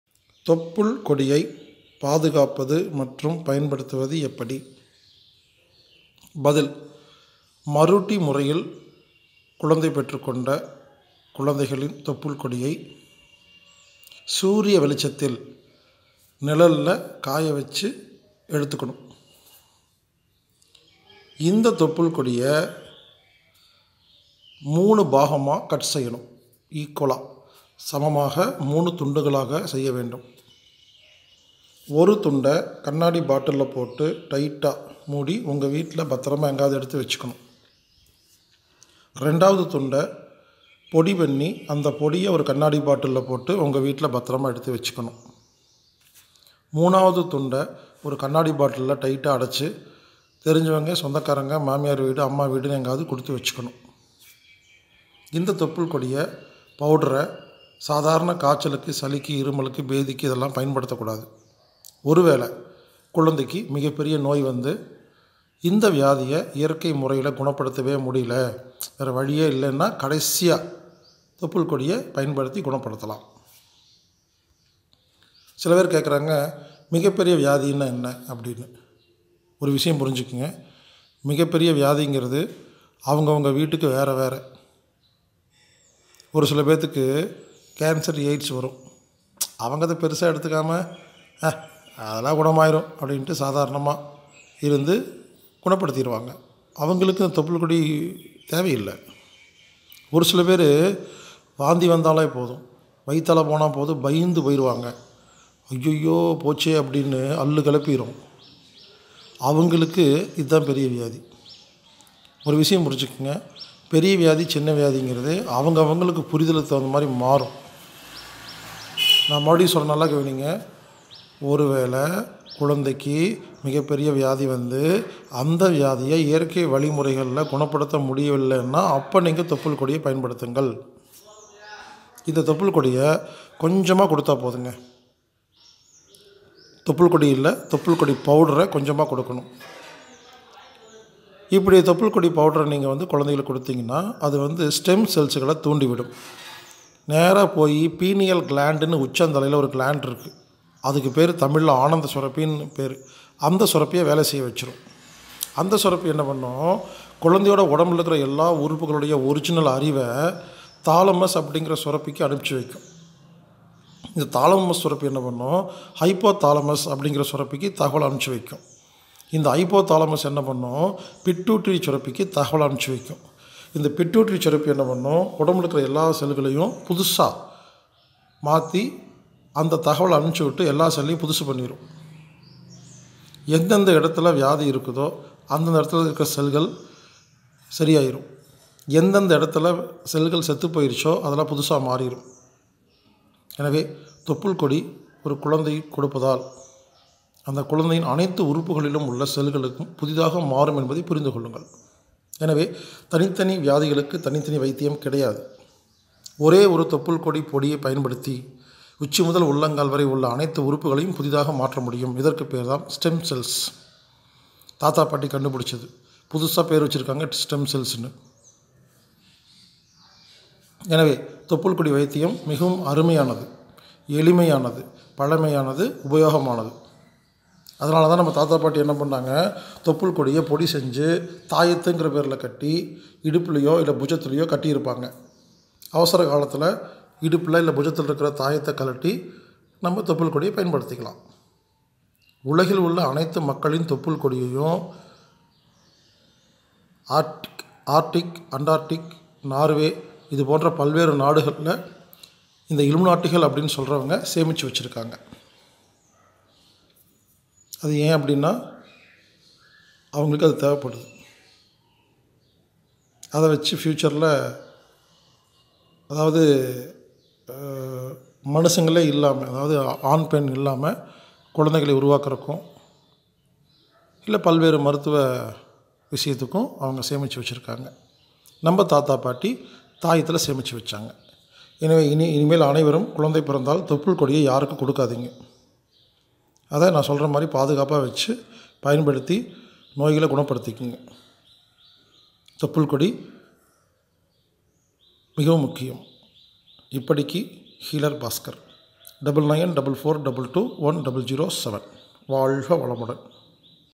தொப்புள் கொடியை பாதுகாப்பது மற்றும் பயன்படுத்துவது எப்படி(QN: 229) Healer Baskar
கேள்வி எண் : 229 : தொப்புள் கொடியை பாதுகாப்பது மற்றும் பயன்படுத்துவது எப்படி?

பதில் : மரூட்டி முறையில் பெற்ற குழந்தையின் தொப்புள் கொடியை, சூரிய வெளிச்சத்தில் நிழலில் காய வைக்க வேண்டும்.

அந்த தொப்புள் கொடியை மூன்றாக கட் செய்ய வேண்டும்.

ஒன்றை கண்ணாடி பாட்டிலில் அடைத்து உங்கள் வீட்டில் பாதுகாப்பாக வைக்க வேண்டும்.

இரண்டாவதை பொடிசெய்து, ஒரு கண்ணாடி பாட்டிலில் அடைத்து உங்கள் வீட்டில் வைத்துக்கொள்ள வேண்டும். ஏனென்றால் அவசரத்திற்கு பொடி செய்வதற்கு நேரமிருக்காது.

மூன்றாவது துண்டை கண்ணாடி பாட்டிலில் அடைத்து வேற ஒரு வீட்டில், அதாவது, அம்மா வீட்டில், சொந்தக்காரர்கள் வீட்டில் அல்லது நண்பர்கள் வீட்டில் கொடுத்து பாதுகாப்பாக வைத்துக்கொள்ள வேண்டும்.

சாதாரண காய்ச்சலுக்கு, சளி, இருமல், பேதிக்கு இதை பயன்படுத்தக்கூடாது.

ஒருவேளை, குழந்தைக்கு மிகப்பெரிய வியாதி வந்து, எந்த ஓரு இயற்கை வழிகளிளும் குணப்படுத்த முடியவில்லை என்றால் மட்டுமே தொப்புள் கொடியை பயன்படுத்த வேண்டும்.

அந்தப் பொடியை மிகவும் குறைவாக எடுத்து நாட்டு மாட்டு பசும்பாலில் அல்லது லேசாக சூடு செய்த தண்ணீரில் கொஞ்சமாக கலந்து குழந்தைக்கு கொடுக்க வேண்டும்.

ஒரு முறை கொடுத்தால் போதும் அது வேலை செய்யும்.

இப்படி கொடுக்கப்படும் தொப்புள் கொடியின் பொடி குழந்தையின் உடலுக்குள் சென்று ஸ்டெம் செல்களை தூண்டி விடும். உடனே பீனியல் கிளாண்ட் ( PENIAL ) எனப்படும் ஆனந்த சுரப்பி வேலை செய்யும்.

இந்த ஆனந்த சுரப்பியில் உள்ள குழந்தையின் உறுப்புகள் சம்பந்தப்பட்ட ஒரிஜினல் அறிவை தாலமஸ் என்ற சுரப்பிக்கு தகவல் அனுப்பு.

தாலமஸ் சுரப்பி ஹைபோதாலமஸ் என்ற சுரப்பிக்கு தகவல் அனுப்பும். ஹைபோதாலமஸ் என்ற சுரப்பி பிட்யூட்டரி சுரப்பிக்கு தகவல் அனுப்பும். பிட்யூட்டரி சுரப்பி உடம்பில் பாதிக்கப்பட்ட செல்களை மீண்டும் புதிய செல்களாக மாற்றுவதற்கான தகவலை அனுப்பவும்.

அதன் பிறகு உடலில் எந்த எந்த இடத்தில் நோய் உள்ளதோ அல்லது எந்த எந்த இடத்தில் செல்கள் இறந்து விட்டதோ அந்த செல்கள் புதிதாக மாறும்.

எனவே தொப்புள்கொடி பொடியை ஒரு குழந்தைக்கு கொடுப்பதால் அந்த குழந்தையின் அனைத்து உறுப்புகளில் உள்ள செல்களும் புதிதாக மாறும் என்பதை புரிந்து கொள்ளுங்கள்.

எனவே தனித்தனி வியாதிக்கு தனித்தனி வைத்தியம் கிடையாது. ஒரே ஒரு தொப்புள் கொடி பொடி, உச்சி முதல் உள்ளங்கால் வரை அனைத்து உறுப்புகளை புதிதாக மாற்றி நோய்களை குணப்படுத்தும்.

எனவே, தொப்புள்கொடி வைத்தியம் மிகவும் அருமையானது, எளிமையானது, பழமையானது உபயோகமானது.

எனவேதான் நமது முன்னோர்கள் தொப்புள்கொடியை பொடிசெய்து, தாயத்து என்ற பெயரில் இடுப்பிலோ அல்லது புஜத்திலோ  கட்டி இருப்பார்கள்.

அவசர காலத்திற்கு இடுப்பில் அல்லது புஜத்தில் உள்ள தாயத்தை கழட்டி அதிலுள்ள தொப்புள்கொடி பொடியை பயன்படுத்துவார்கள்.

உலகிலுள்ள அனைத்து மக்களின் தொப்புள் கொடியையும்,  நார்வே பகுதியில்,  இஸ்ரேல் நாட்டைச் சார்ந்த யூத இலுமினாட்டிகள் சேமித்து வைத்திருக்கிறார்கள்.

இப்படிக்கு.